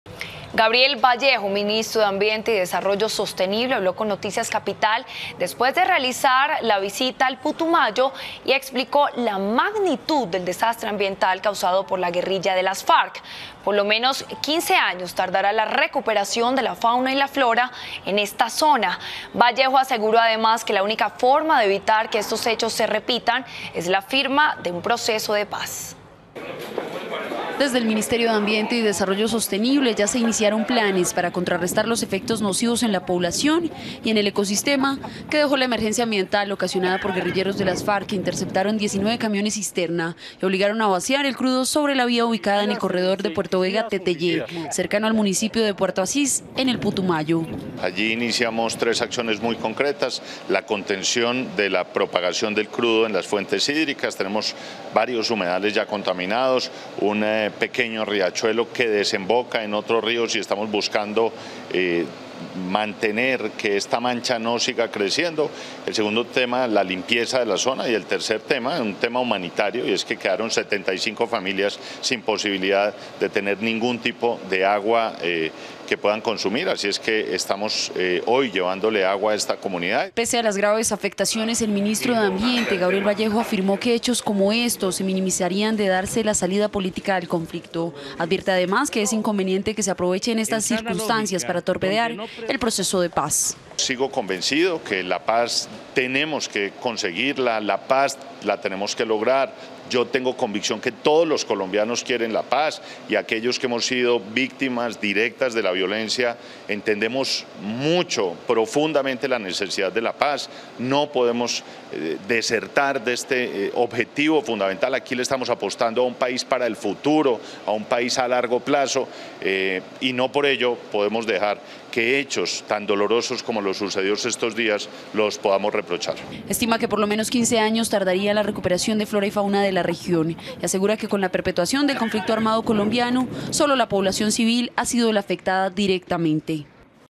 Gobierno asegura que daño ambiental causado por Farc en Putumayo tardará 15 años para recuperarse
Gobierno asegura que el daño ambiental causado por las Farc en el Putumayo  tardará 15 años para recuperarse

Noticias Capital 7 p.m. 11 de junio 2015